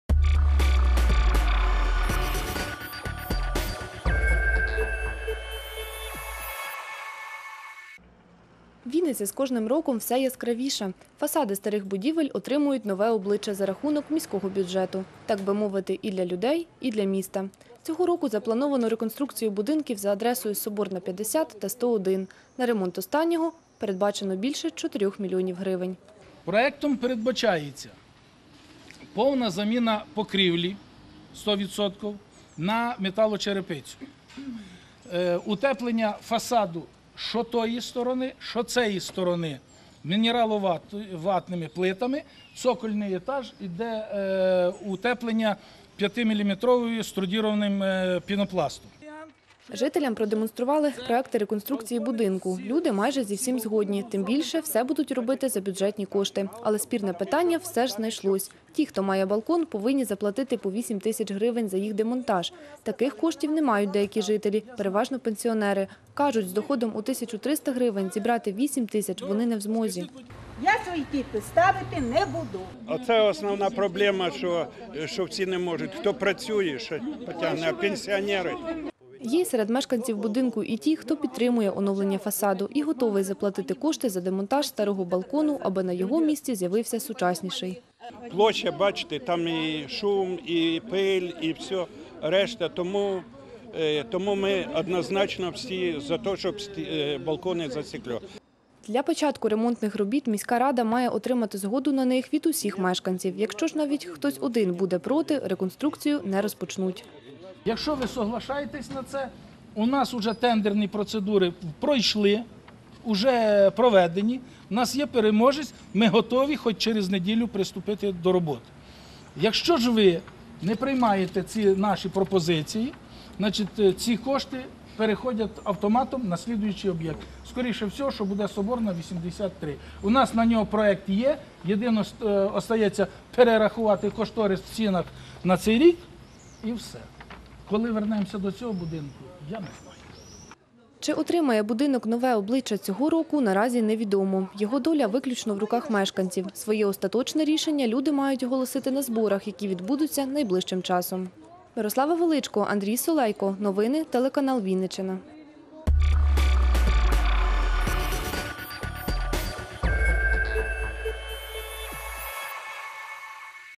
Реконструкція будинків у центрі міста 08 06 2017
Міська рада хоче покращити естетичний вигляд будівель у центрі міста. Цього року черга дійшла до будинку на Соборній 101. Кошти на реконструкцію виділили, проте роботу розпочати не можуть,адже не всі жителі дають згоду. Аби вирішити це питання організували збори. Чи вдалося домовитись мешканцям будинку та чиновникам - далі у сюжеті.